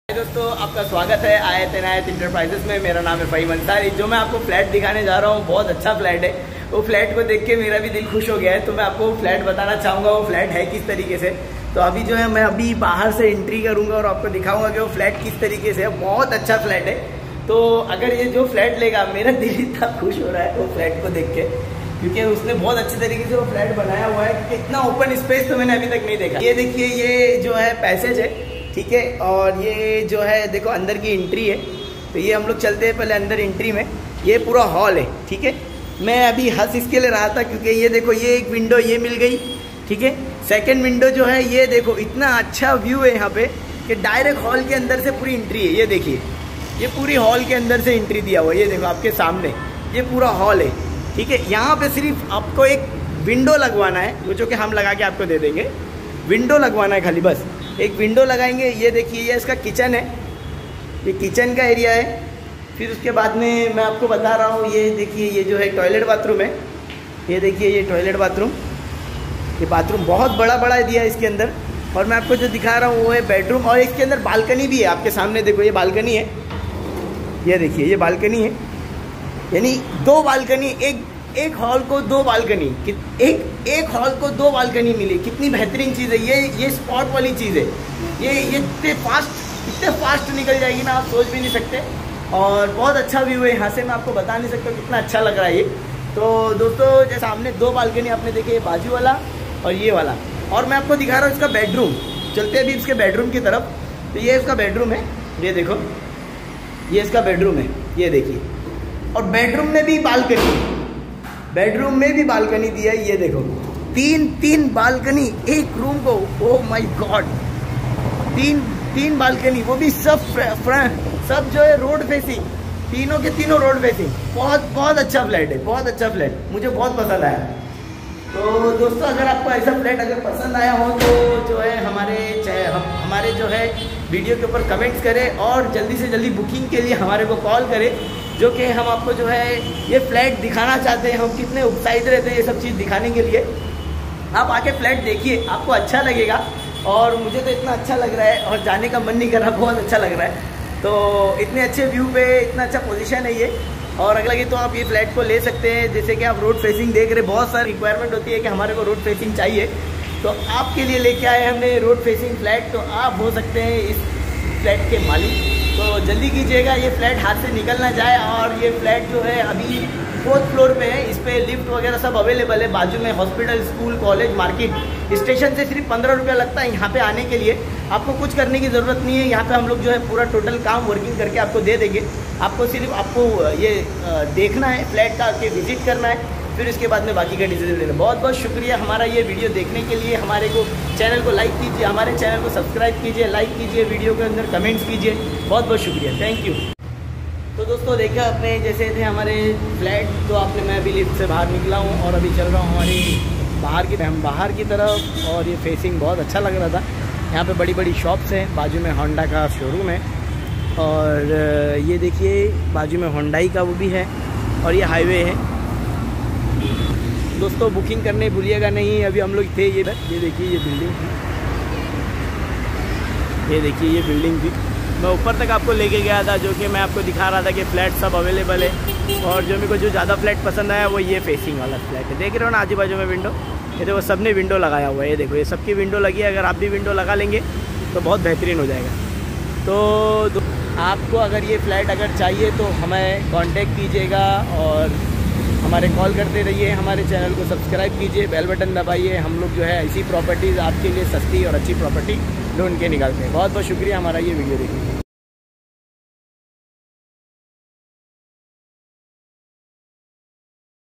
DOMBIVALI 630sq feet 3 balconies Road Facing Flat / 7208444559
1bhk FLAT 630sq feet with 3 balconi Road Facing Flat
BOOKING 51000/-
LIFT MARKET ATM SCHOOL COLLEGE SONARPADA